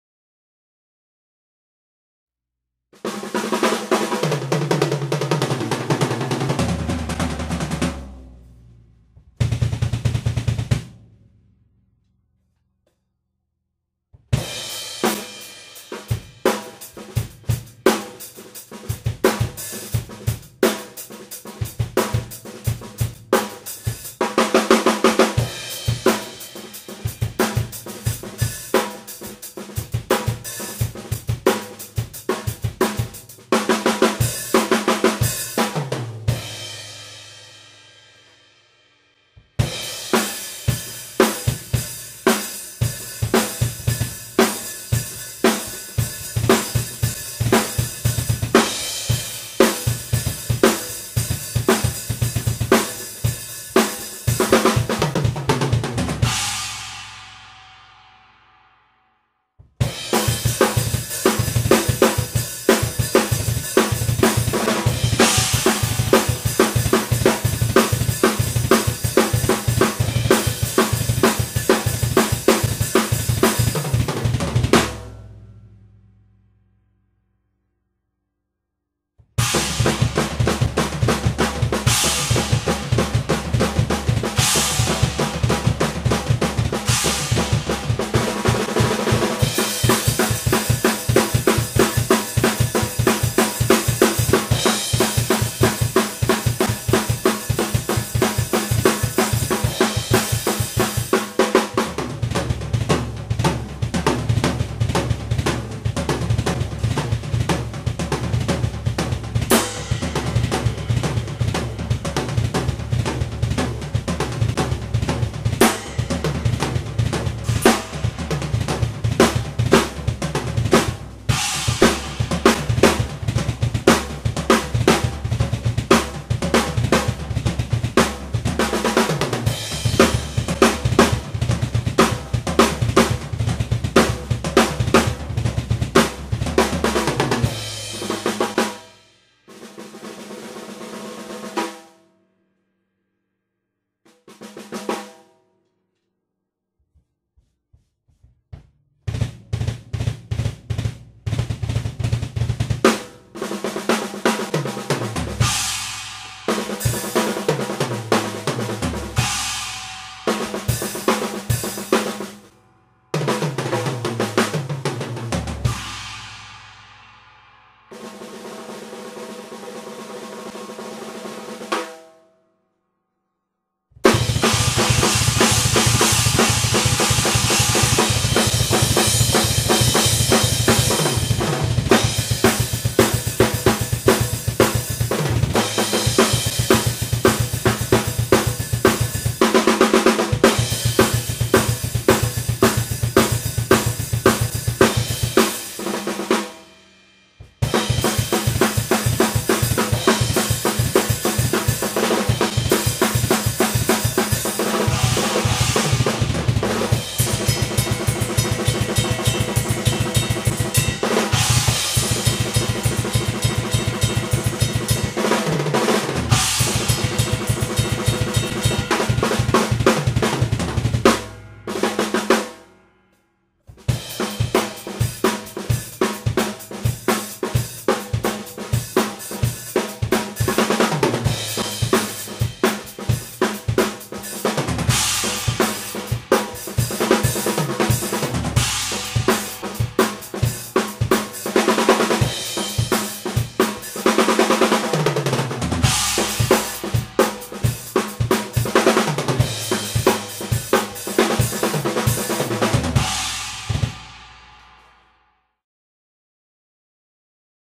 Tama Starclassic Bubinga Select (FULL HD)
Just messing around on my drumkit!
Tama Starclassic Bubinga Select , marbled red metallic.
Pork Pie Rosewood Zebra Maple 13x7''
Zildjian cymbals.
Recorded with Zoom Q4 HD. Settings: Gain - Low-manual, LO cut - Off